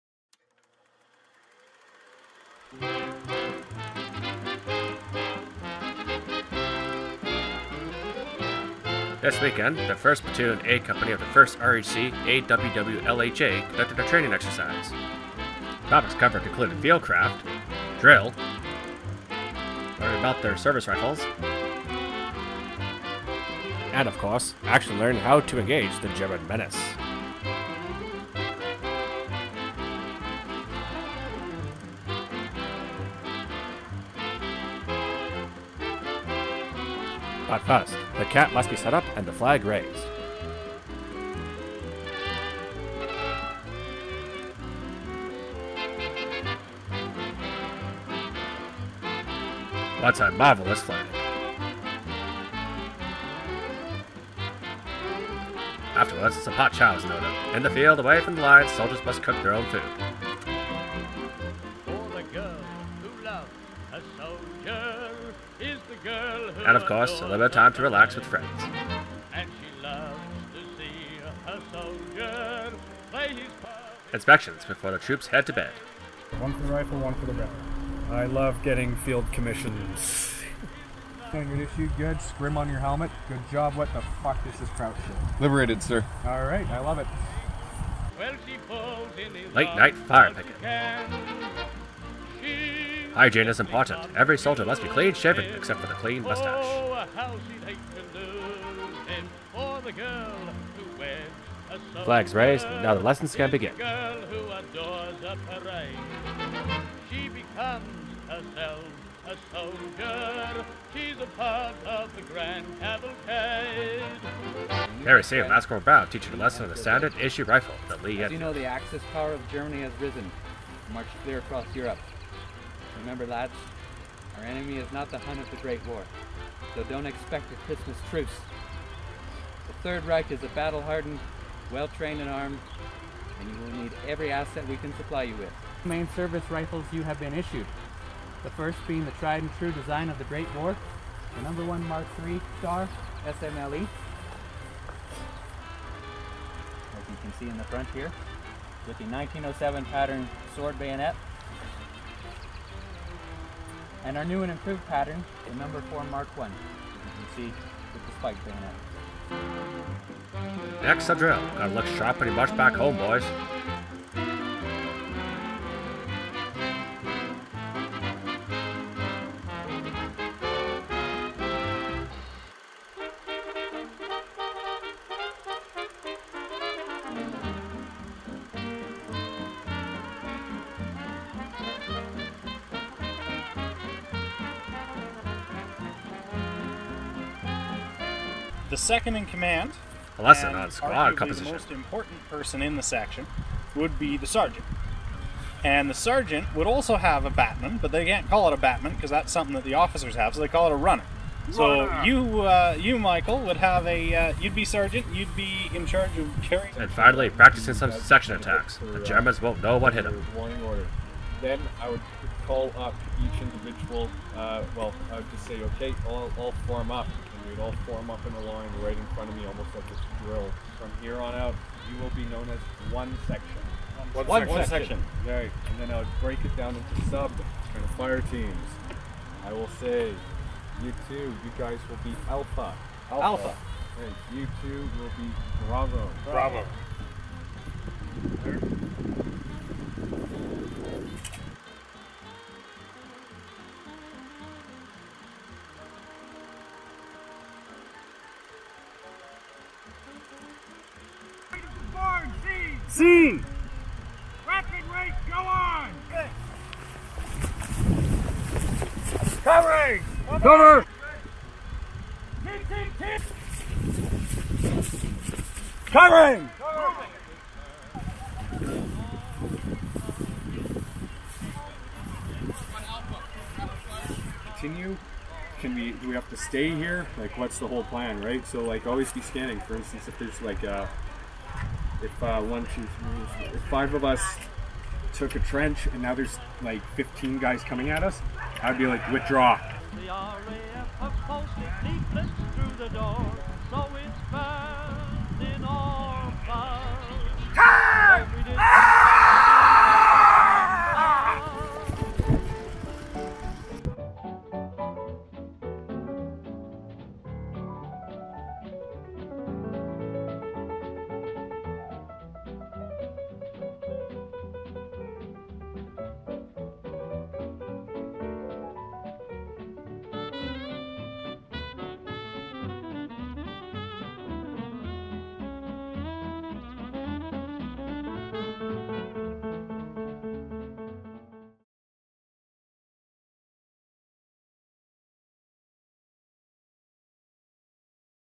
AWWLHA Training Event June 3-5th 2022
The AWWLHA conducted a training event on the weekend of June 3rd-5th 2022. The Military Museums were gracious enough to allow us to use their facility for the event. We had a blast interacting with the public and conducting our training. We look forward to seeing everyone at Summer Skirmish 2022!